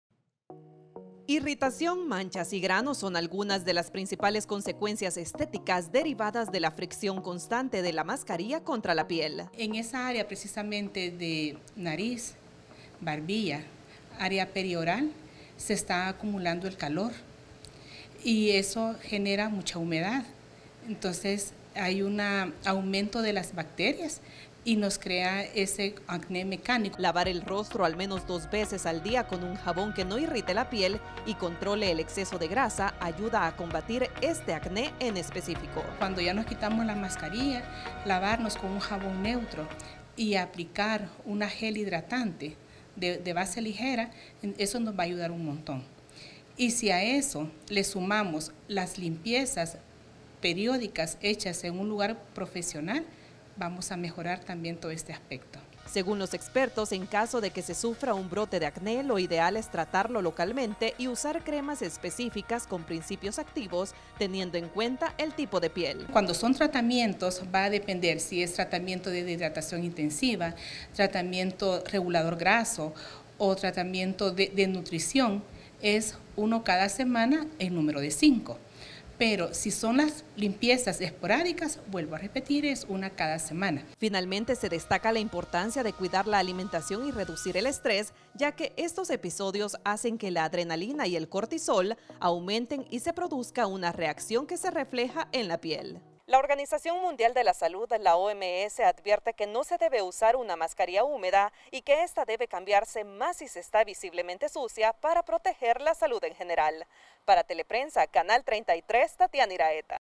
Teleprensa33 | Acné mecánico provocado por uso de mascarilla
Utilizar mascarilla para evitar el Covid-19 no implica descuidarse de la piel.